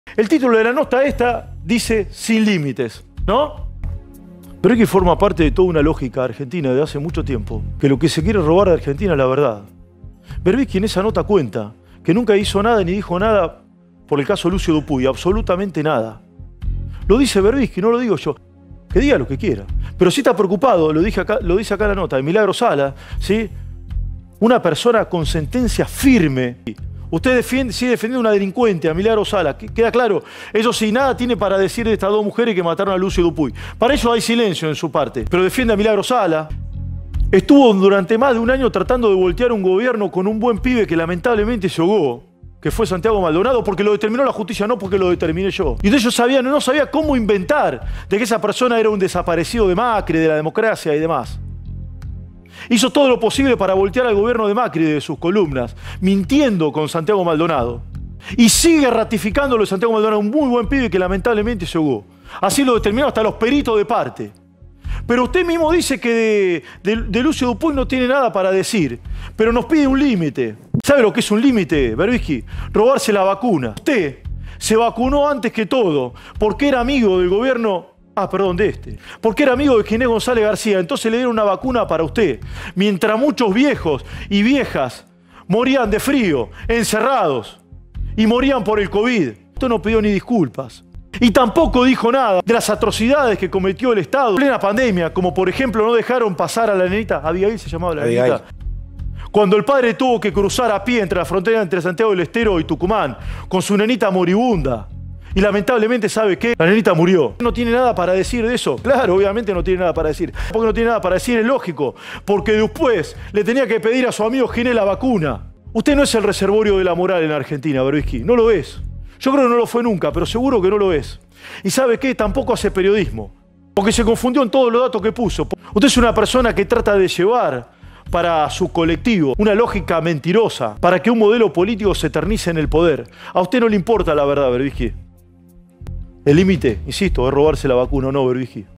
El pelado Trebucq demolió al zurdo Verbitsky.
El pelado Trebucq demolió al zurdo Verbitsky. 2 minutos épicos para mirarlo un millón de veces.  👊👊👏👏